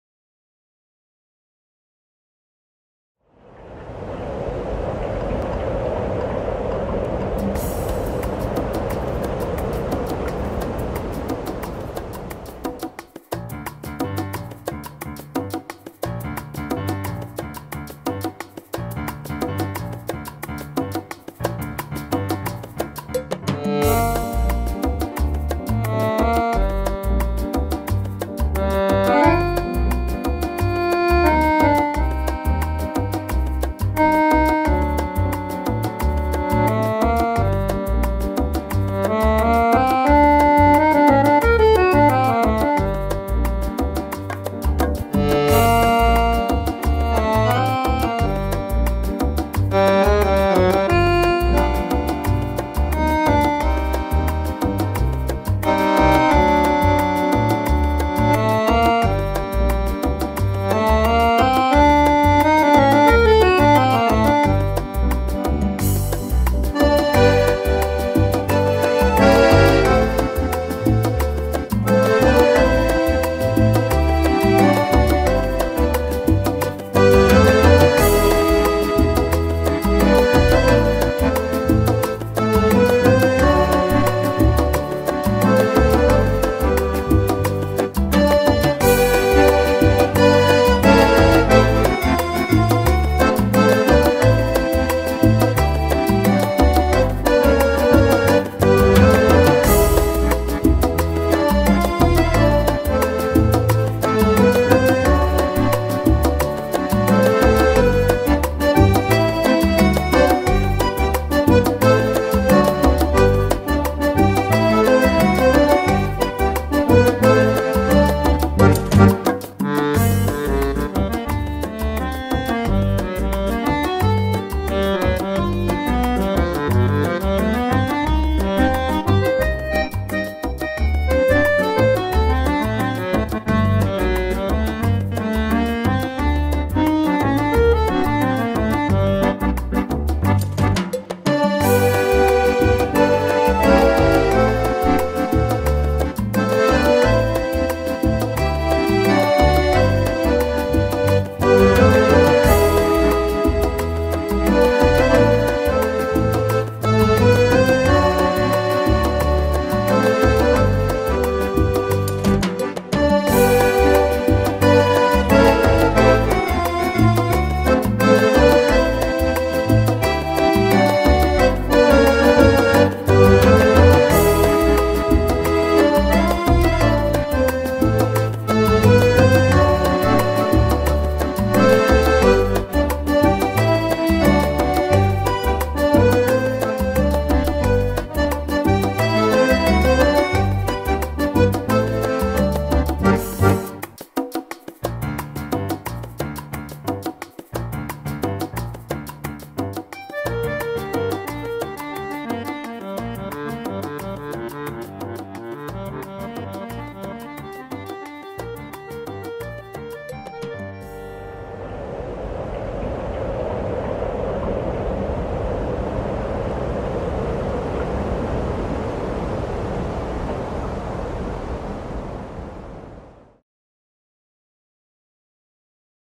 EnCasa con Maricarmen Vázquez
Enjoy this special recording from the talented accordionist Maricarmen Vázquez!